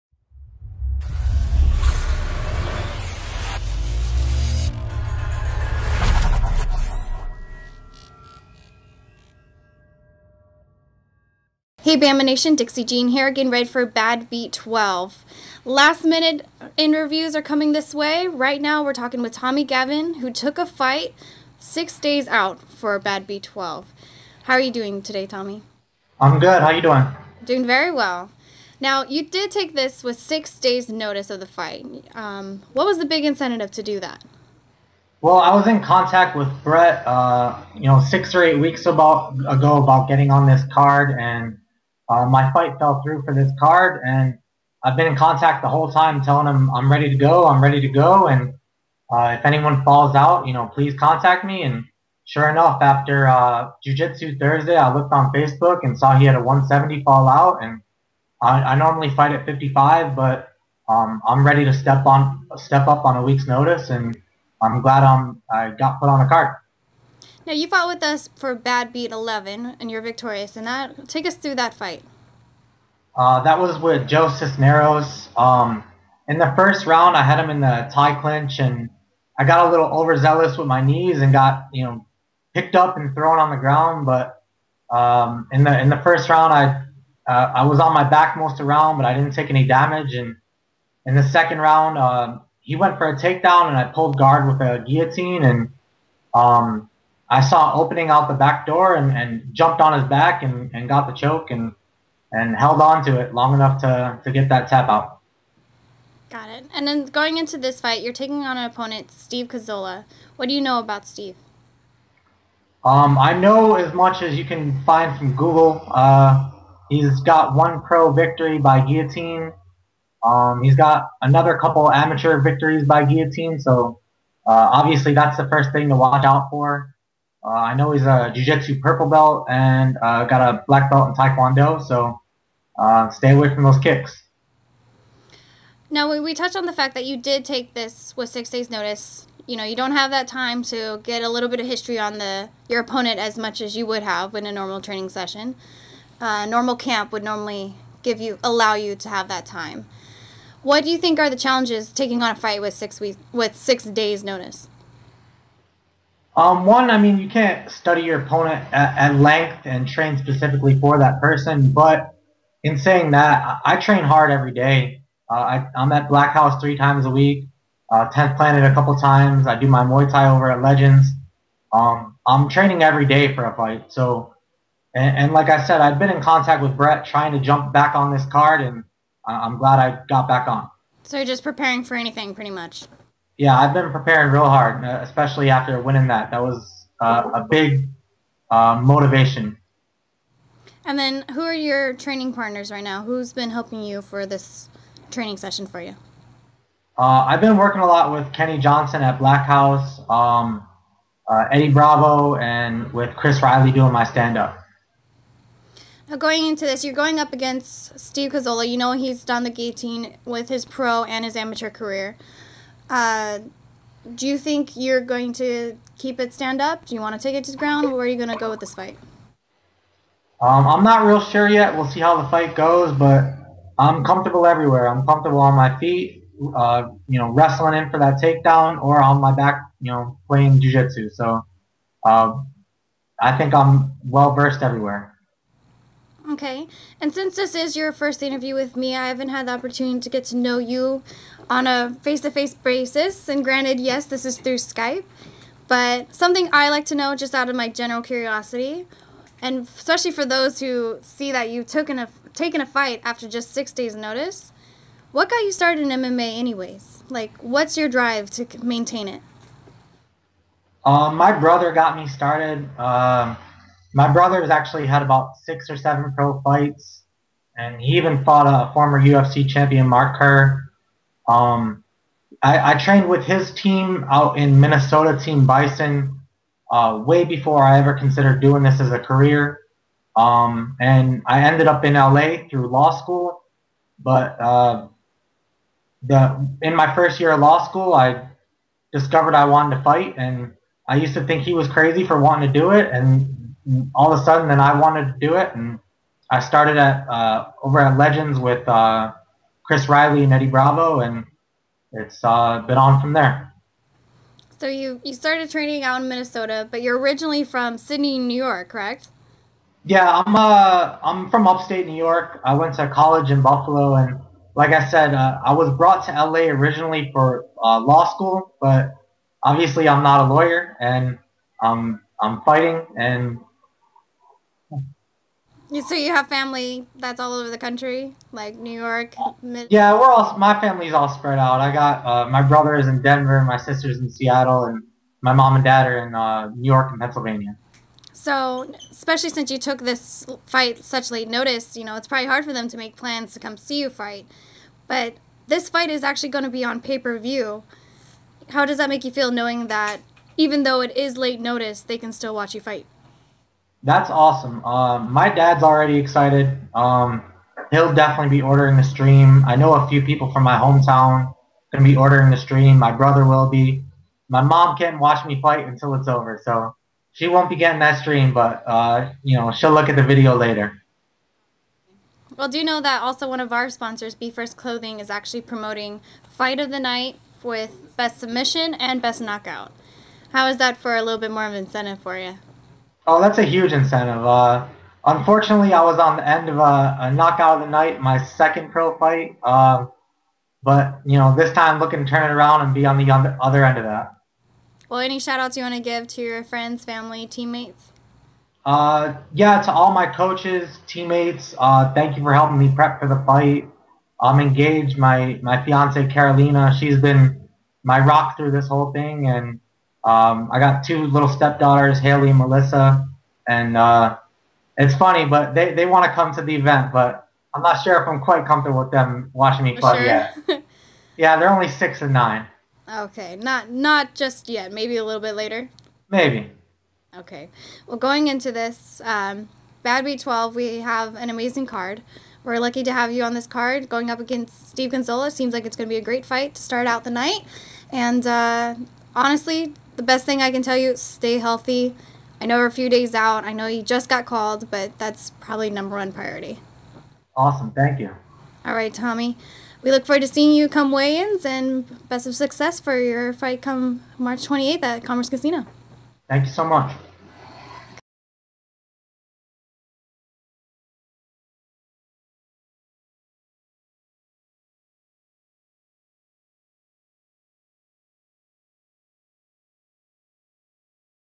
Tommy Gavin Badbeat 12 Pre Fight Interview
BAMMA USA will hold its first event of 2014 with Badbeat 12, presented by Budweiser. Badbeat 12 takes place at the world famous Commerce Casino on Friday, March 28, 2014. Doors open at 6PM. Fights start promptly at 7PM PST. For fans that cannot make it to the live event, Badbeat 12 will be streamed on Go Fight Live.TV (GFL) pay-per-view (PPV).
Badbeat 12 card will feature a catchweight bout between Tommy Gavin (1-2) and Steve Kozola (1-0). Gavin will look to build on his last fight at Badbeat 11, where he earned the first professional win of his career by defeating Joe Cisneros via submission (rear-naked choke). The 10th Planet, Legends, and Blackhouse product Gavin accepted the fight against Steve Kozola on eight days notice, and will be the opening fight of a star-studded Badbeat 12 card.
Check out this Badbeat 12 pre-fight interview with Tommy Gavin as he talks with Dixie Jean about his upcoming bout with Steve Kozola, explaining why he accepted the fight on short notice, and what the fans can expect on March 28th.